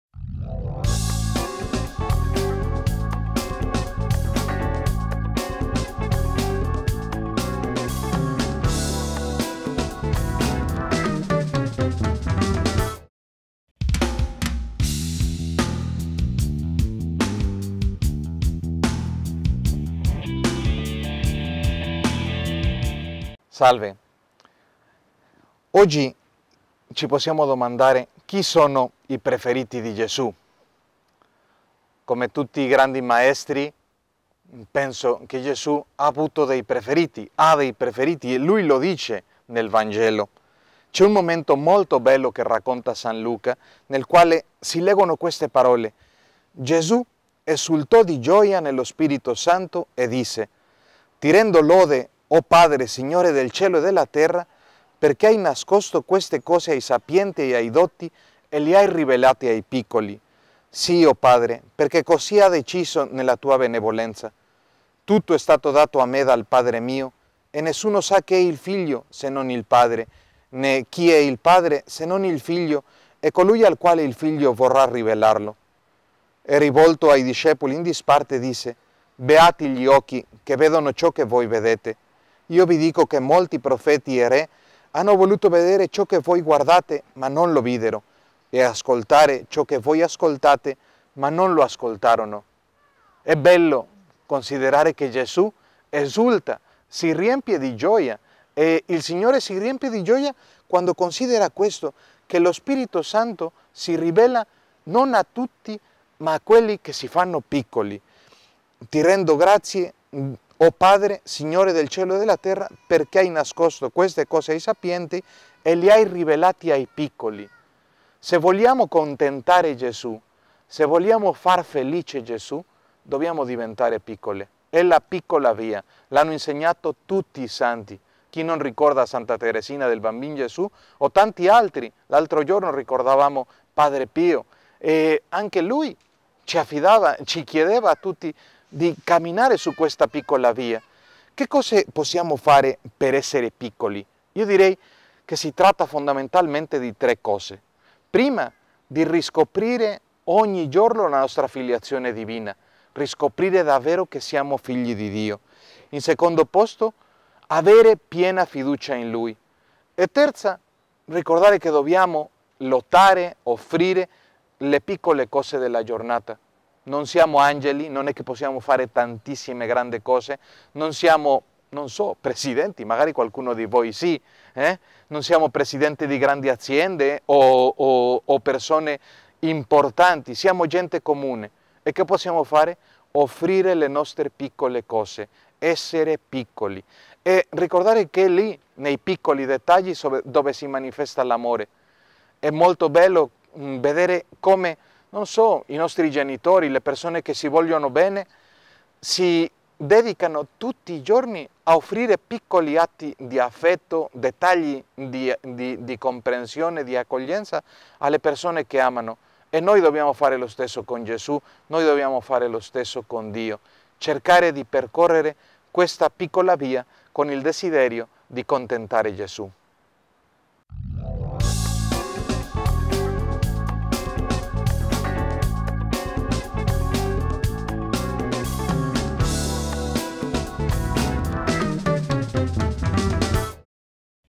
Chi sono i preferiti di Gesù?
Il vangelo del giorno 05/10/2013 risponde a una domanda della nostra vita quotidiana. Oggi chi ci propone una risposta è Don Sergio Tapia Velasco, questa risposta ci aiuta ad andare oltre le domande.